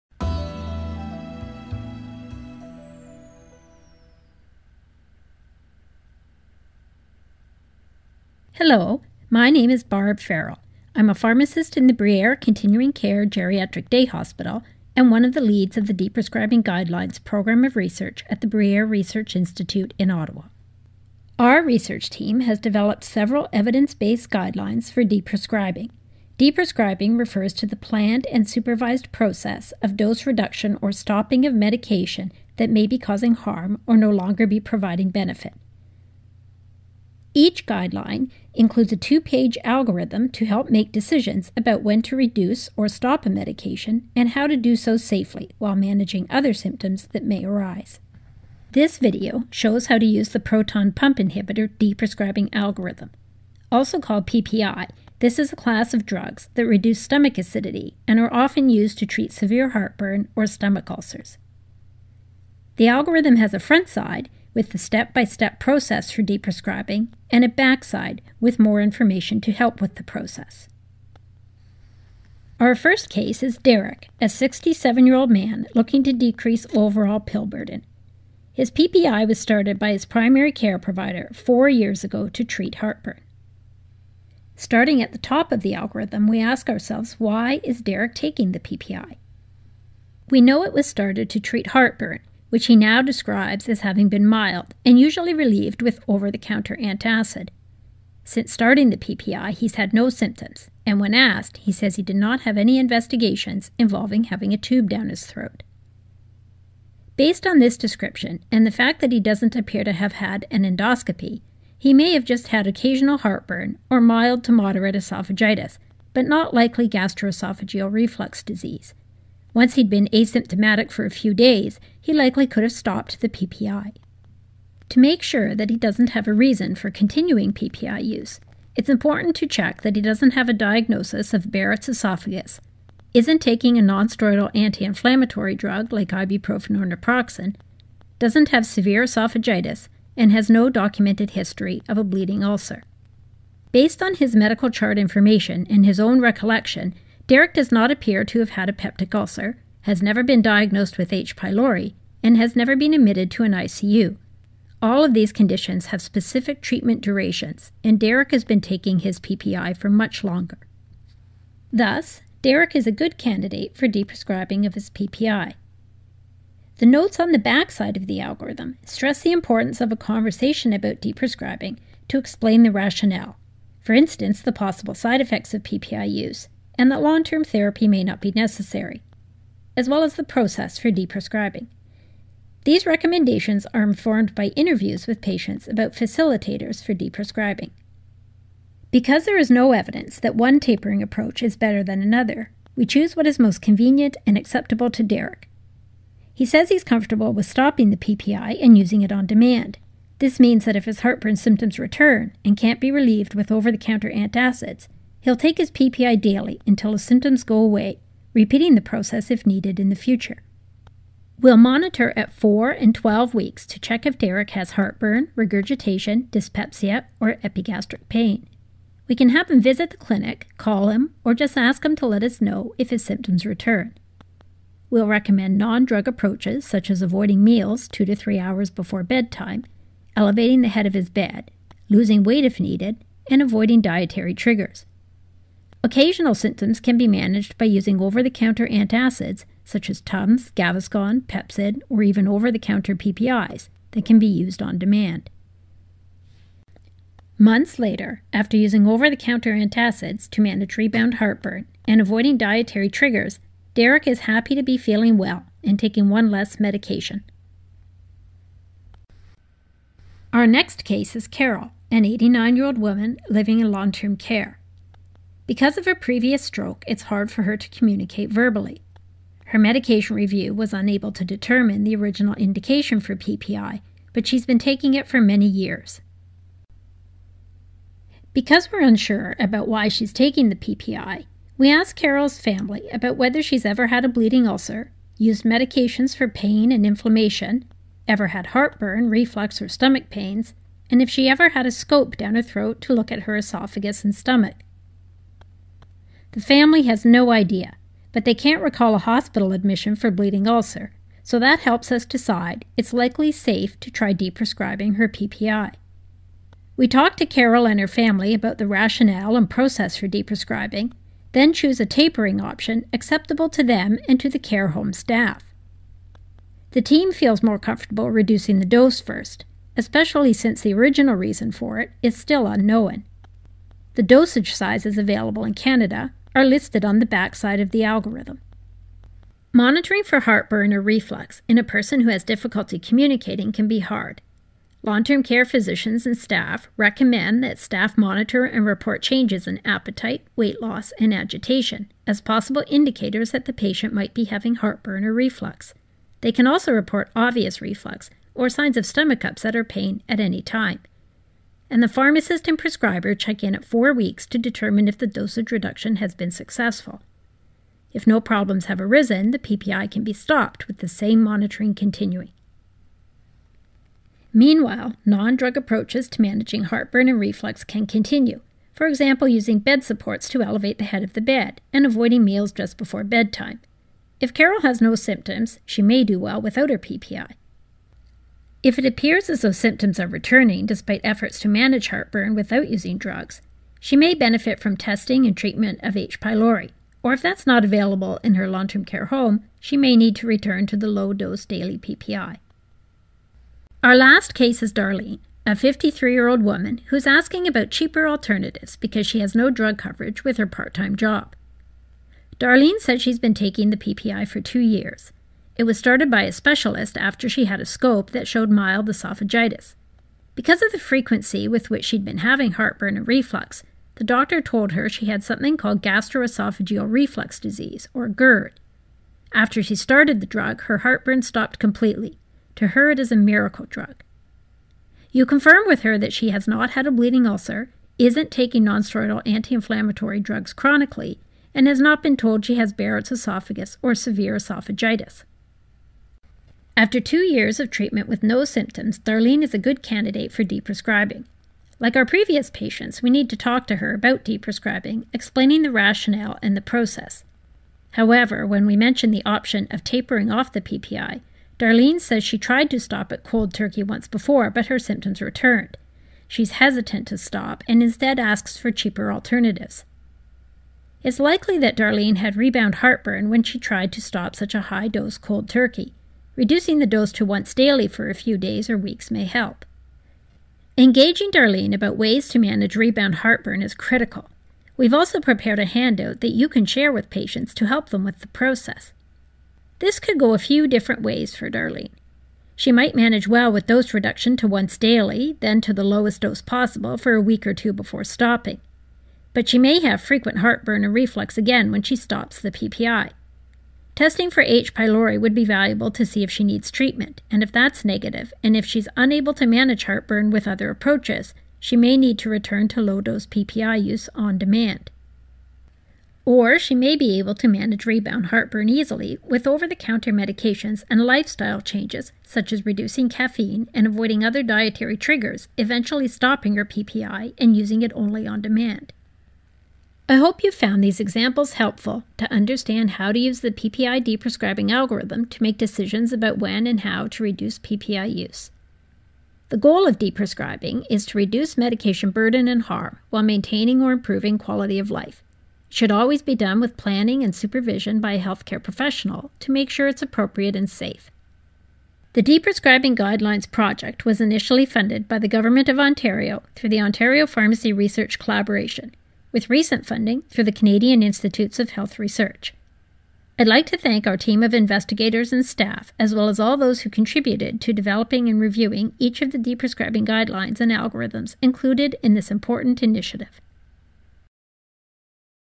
Using the PPI Deprescribing Algorithm - When to Reduce or Stop PPIs and How?
Proton pump inhibitors (PPI) are a class of drugs used to treat heartburn, gastroesphageal reflux disease and gastric ulcers. This video provides an overview of how to use an evidence-based PPI deprescribing algorithm to help you decide when and how to safely reduce or stop PPIs if they are no longer needed, or possibly causing more harm than benefit. It also highlights the situations in which people should continue their PPIs.

Interested in learning more about how we evaluated these videos?